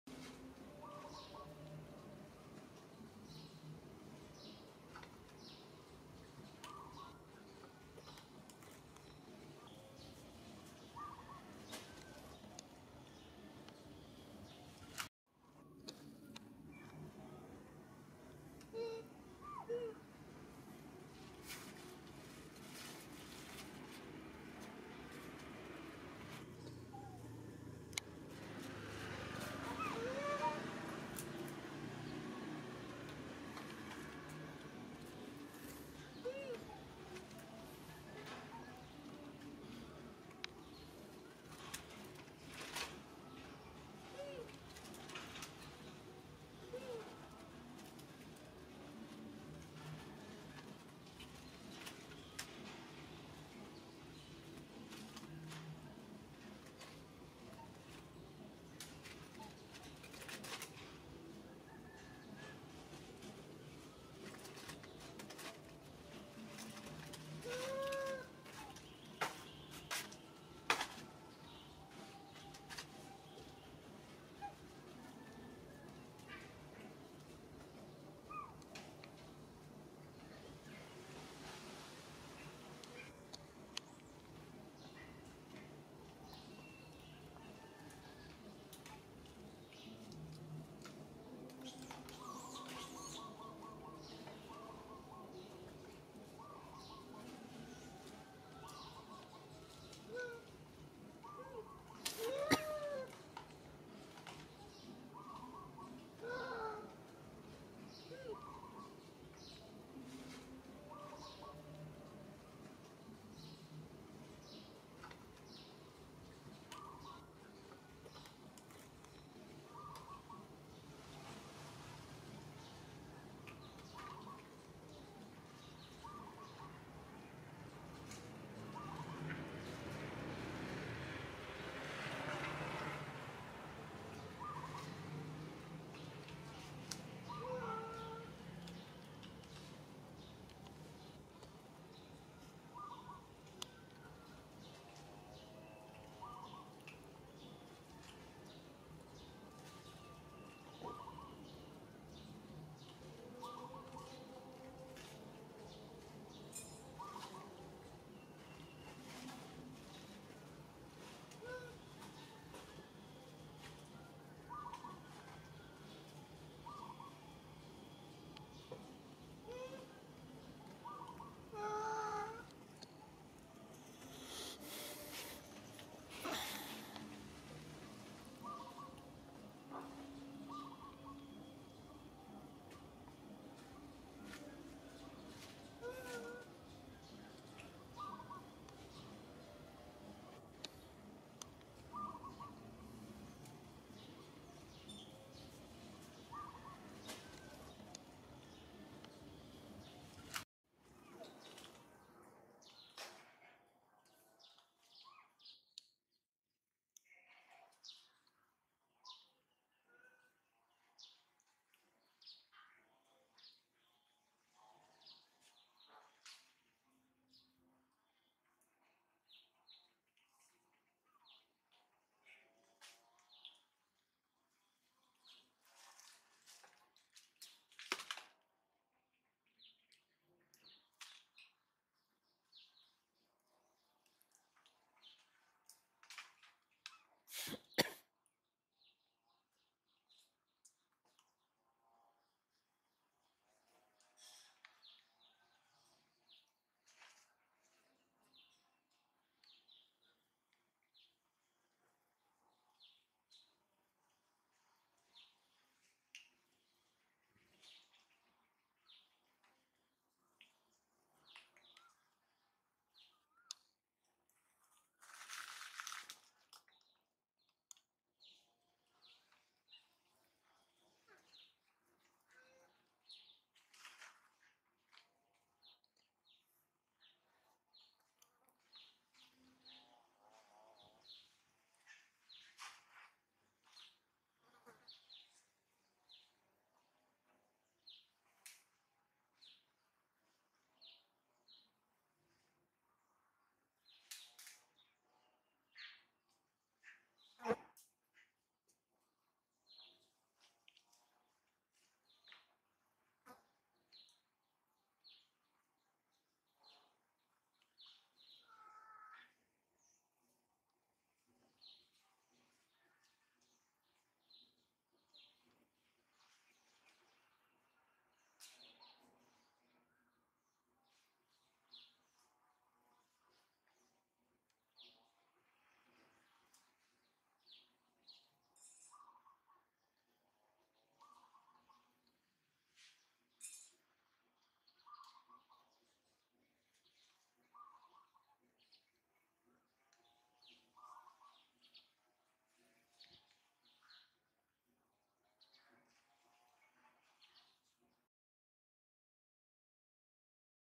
Mother Melona feels sad and full of hungriness, after laboring her birth of baby
Hello everyone welcome Cute Nita Monkey. Today, I would like to share you the story of Mother Melona feels sad and full of hungriness, after laboring her birth of baby.

Thank very much for watching and please help you comment in order to improve my channel.